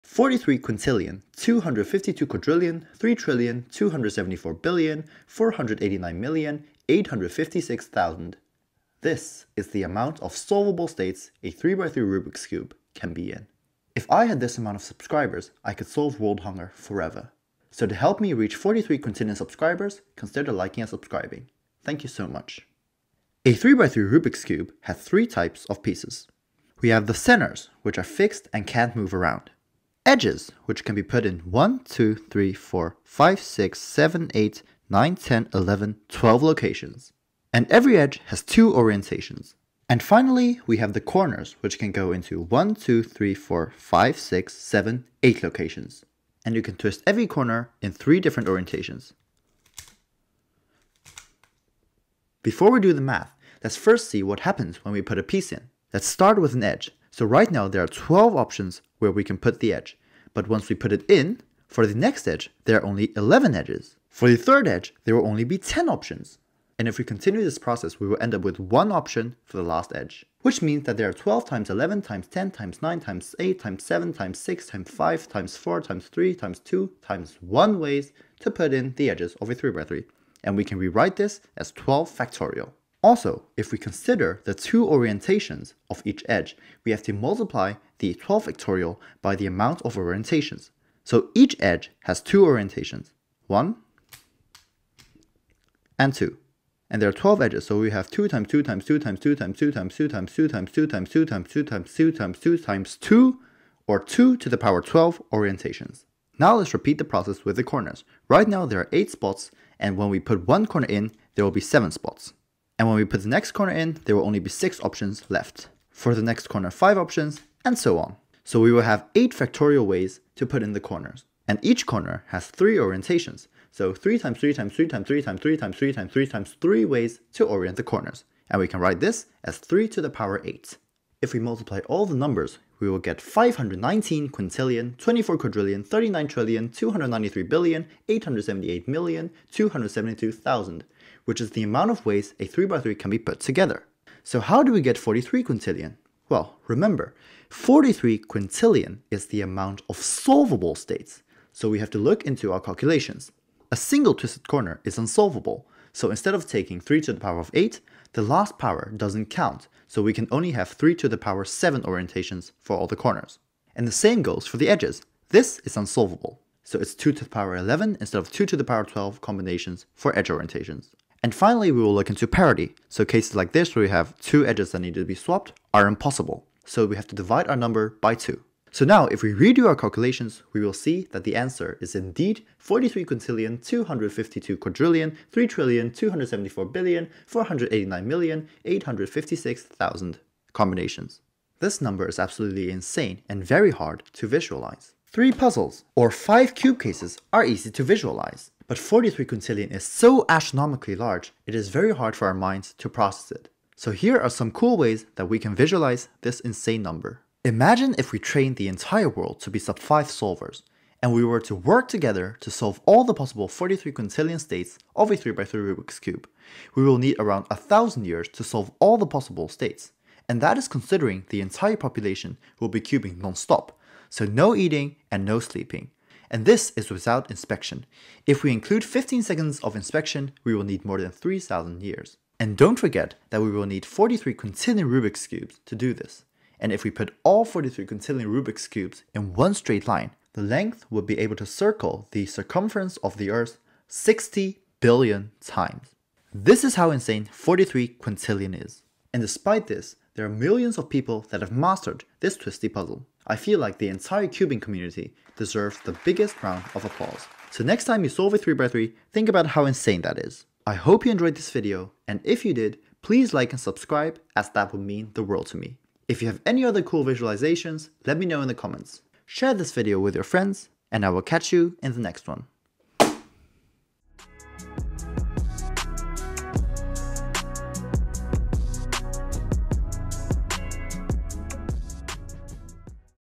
43 Quintillion Rubik’s Cube Combos Explained — It Would Take 1,000 Years to Solve Them All
43,252,003,274,489,856,000 — that’s how many possible combinations a Rubik’s Cube has.

But what does that number actually mean? In this video, I’ll break down where that number comes from, and show you just how insanely massive it really is.

And trust me — even if every cuber on Earth could solve in 5 seconds... it still wouldn’t be enough to go through all of them.

Timestamps:

00:00 Intro
00:24 3x3 Explained
00:52 Calculations
03:12 Visualisations
04:09 Conclusion
04:20 Outro